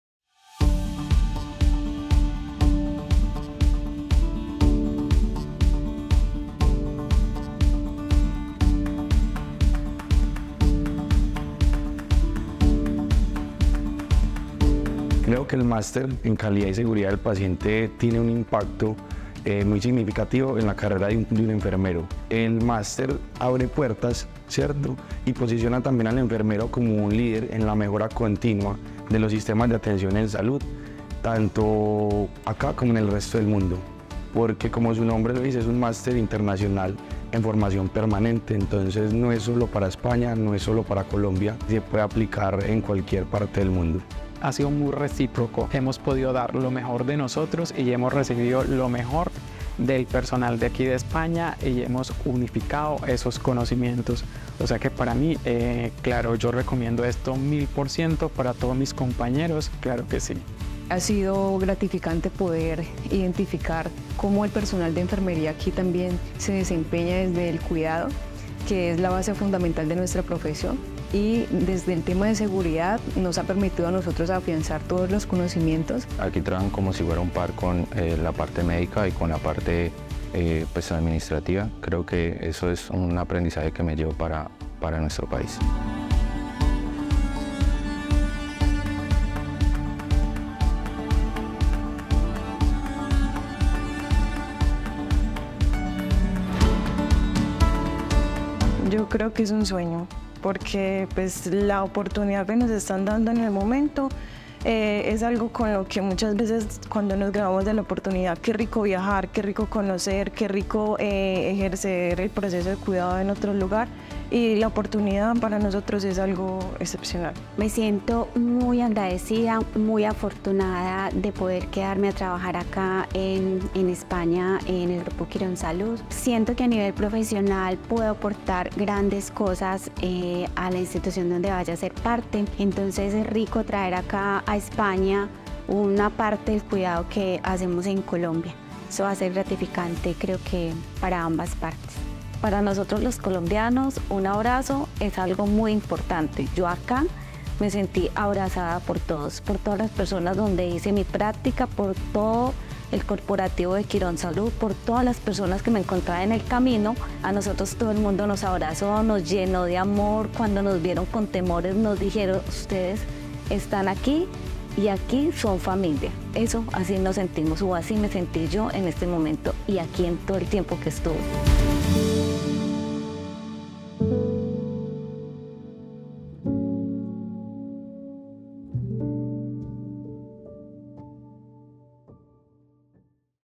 Clausura del I Máster de Formación Permanente en Calidad Asistencial y Seguridad Clínica
Los alumnos del Máster de Formación Permanente en Calidad Asistencial y Seguridad Clínica de Quirónsalud y la Universidad Alfonso X el Sabio concluyen su periodo de prácticas. Algunos de los alumnos pertenecientes a centros de Quirónsalud en Colombia continuarán su desarrollo profesional en centros del Grupo en España.